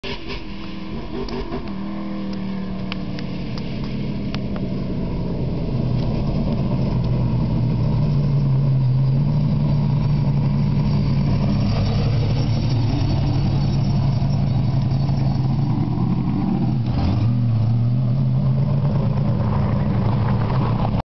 James Hylton Test drive
After servicing the cars out in missouri b/w 2 races, James takes it for a little test drive